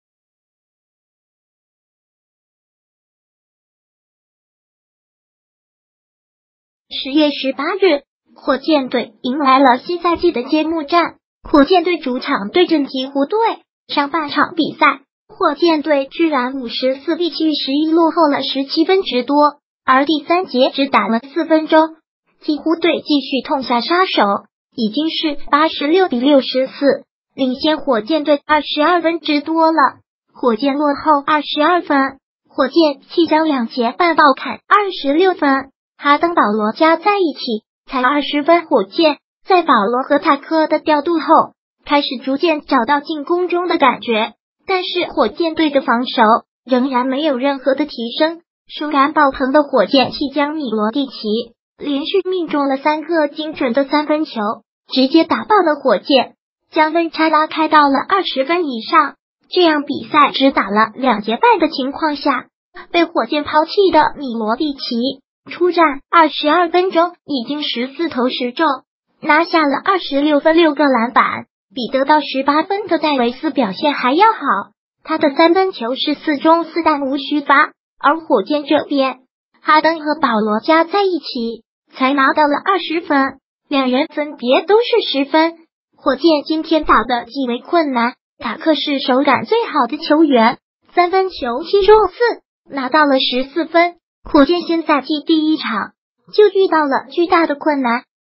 火箭落后22分！火箭弃将两节半爆砍26分，哈登保罗加在一起才20分
10月18日，火箭队迎来了新赛季的揭幕战，火箭队主场对阵鹈鹕队。上半场比赛，火箭队居然54比71落后了17分之多，而第三节只打了4分钟，鹈鹕队继续痛下杀手，已经是86比64，领先火箭队22分之多了！火箭落后22分！火箭弃将两节半爆砍26分，哈登保罗加在一起才20分火箭在保罗和塔克的调度后，开始逐渐找到进攻中的感觉，但是火箭队的防守仍然没有任何的提升，手感爆棚的火箭弃将米罗蒂奇，连续命中了3个精准的3分球，直接打爆了火箭，将分差拉开到了20分以上。这样比赛只打了2节半的情况下，被火箭抛弃的米罗蒂奇，出战22分钟已经14投10中，拿下了26分6个篮板，比得到18分的戴维斯表现还要好，他的3分球是4中4弹无虚发。而火箭这边，哈登和保罗加在一起，才拿到了20分，两人分别都是10分。火箭今天打的极为困难，塔克是手感最好的球员，3分球7中4，拿到了14分。火箭新赛季第一场，就遇到了巨大的困难。